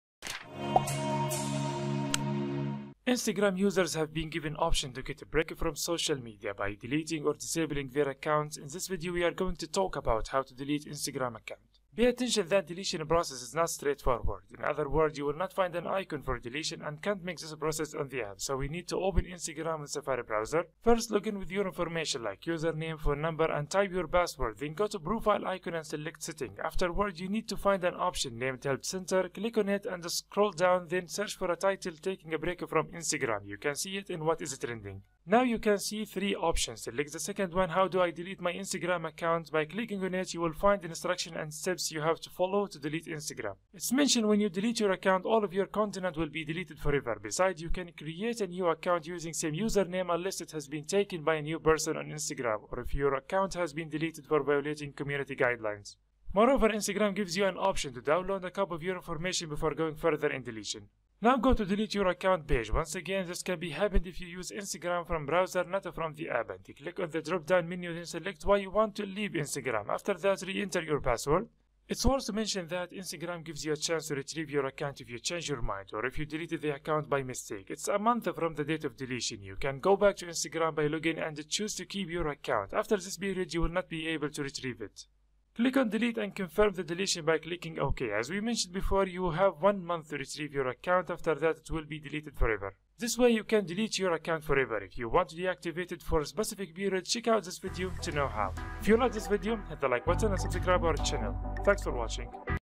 How To Delete an Instagram account permanently on phone - Deletion Page 2022
Instagram users have been given options to get break from social media by deleting or disabling their accounts, in this video we are going to talk about how to delete Instagram account
Pay attention that deletion process is not straightforward ,In other word, you will not find an icon for deletion and can’t make this process on the app
So, we need to open Instagram on Safari browser
First, log in with your information like username, phone number and type your password, then go to profile icon and select settings, afterward you need to find an option named help center, click on it and scroll down, then search for a title “taking a break from Instagram” you can see it in “what is trending”
now you can see three options, select the second one “how do I delete my Instagram account”. By clicking on it , you will find instructions and steps you have to follow to delete Instagram